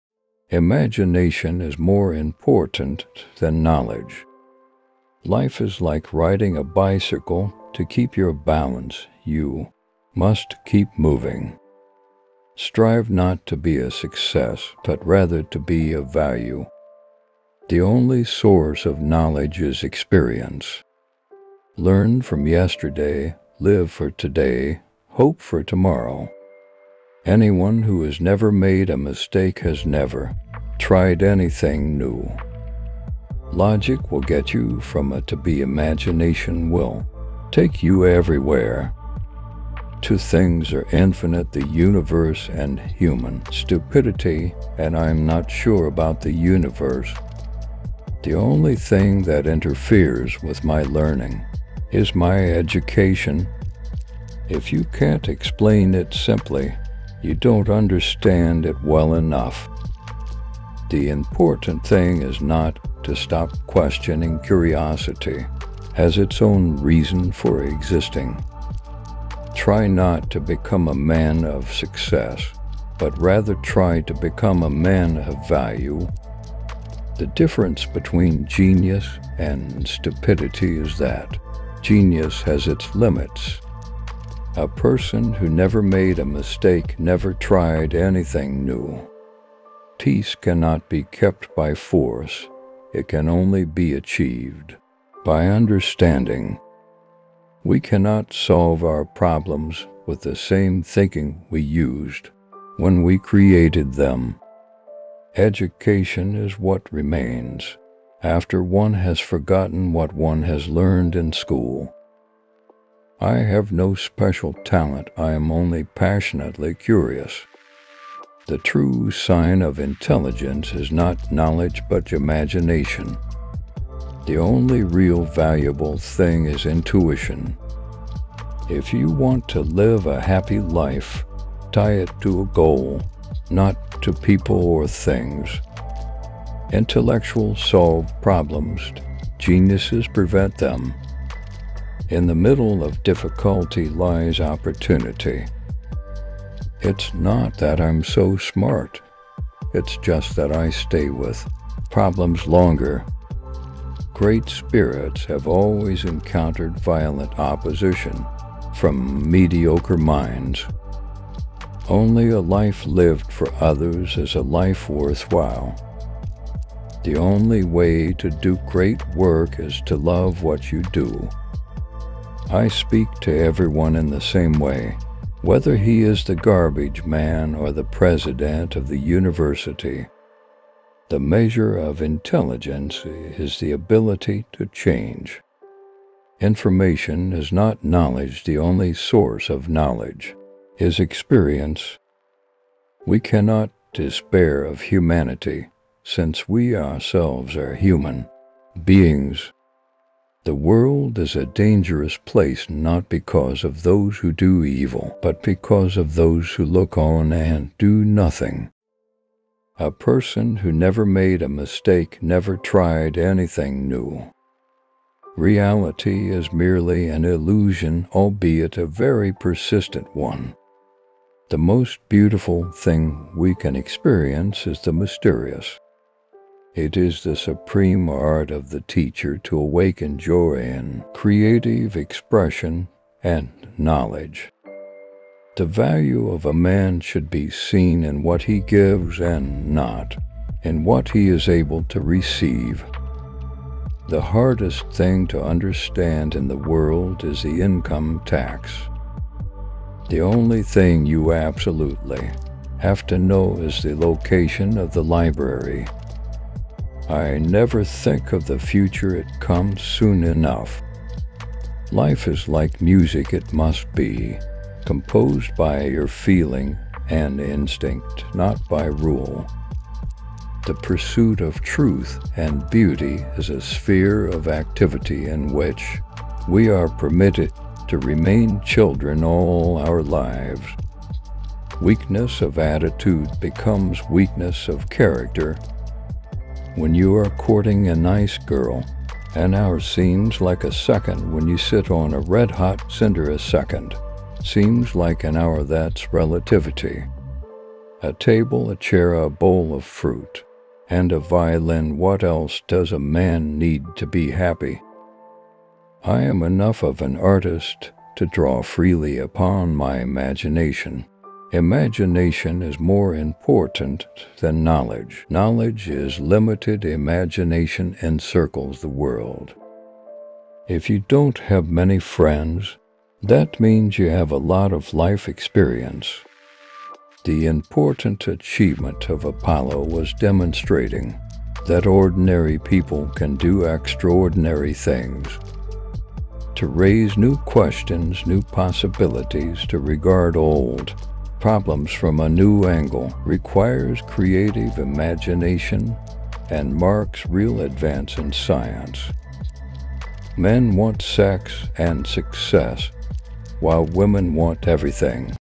Albert Einstein's Life Changing Quotes||Genius Quotes
Albert Einstein was a theoretical physicist and the most famous scientist in human history. He developed the general theory of relativity, one of the two pillars of modern physics, alongside quantum mechanics. He is perhaps best known in popular culture for his mass/energy equivalence formula E=mc2.